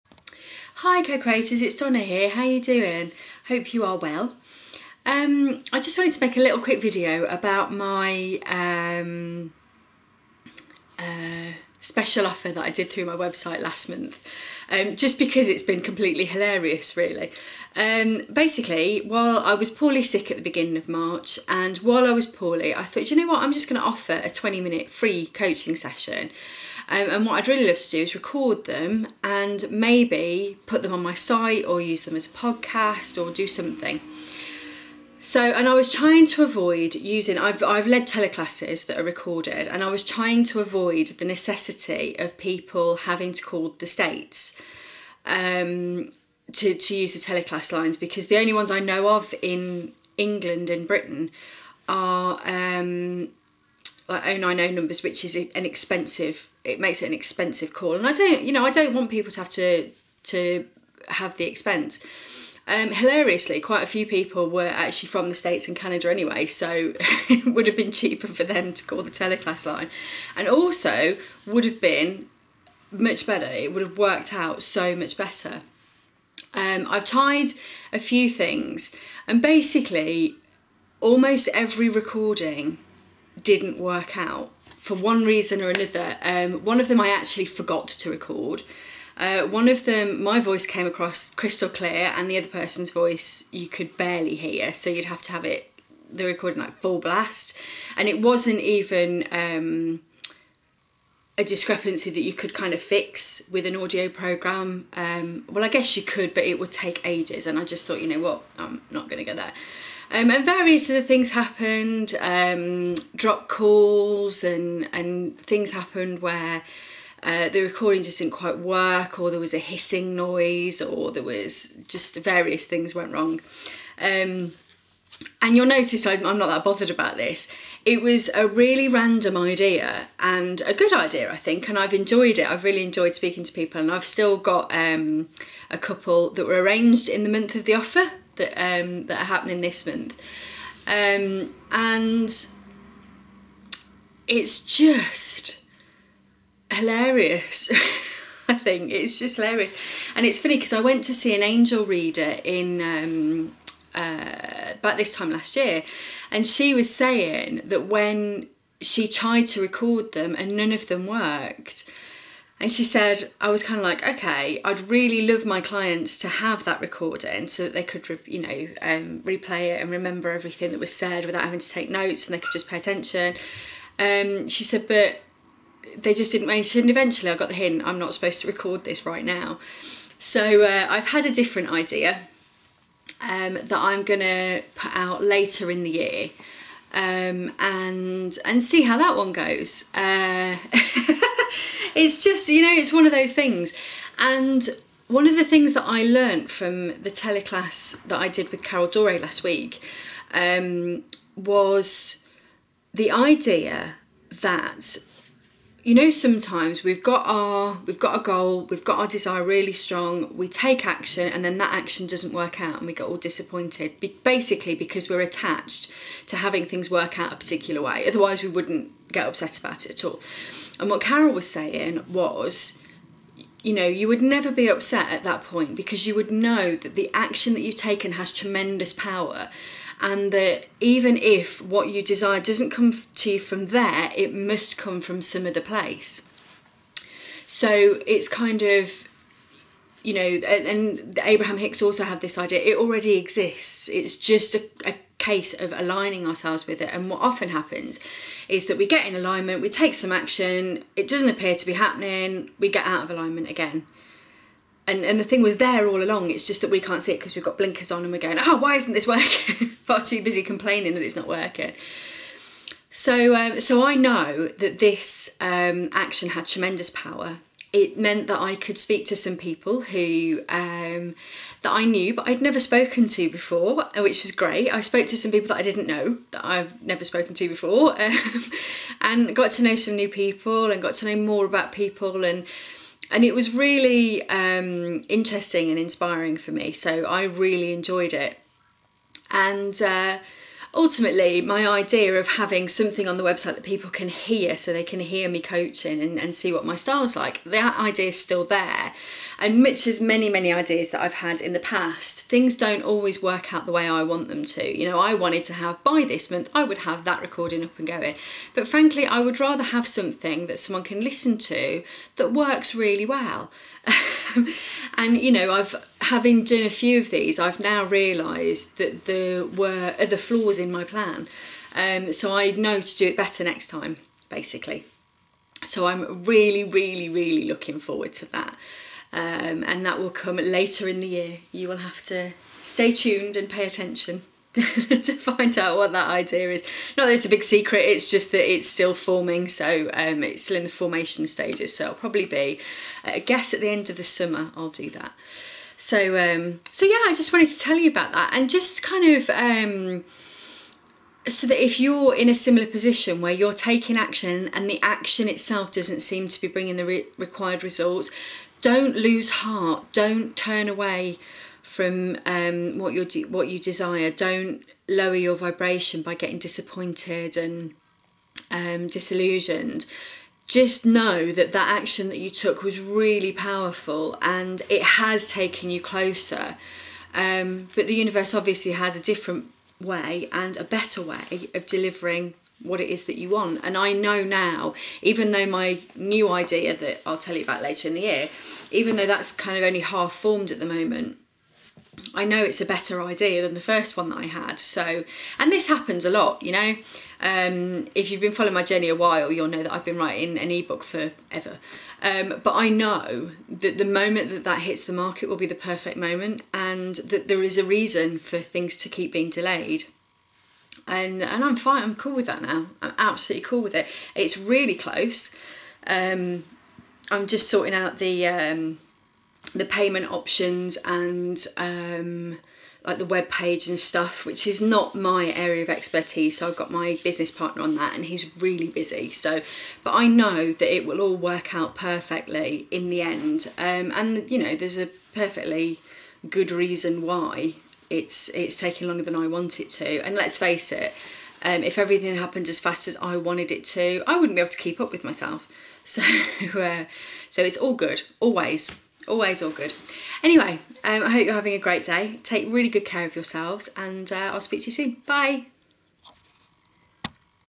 S4 D79 When Things Dont Work Out as you Expect
But, its all good, always.  There is clearly a reason for this palaver  not that I can yet see it!  But I know that not only did I find several ways NOT to do it, I thought of a better way to do it next time, and that the action I took was very powerful.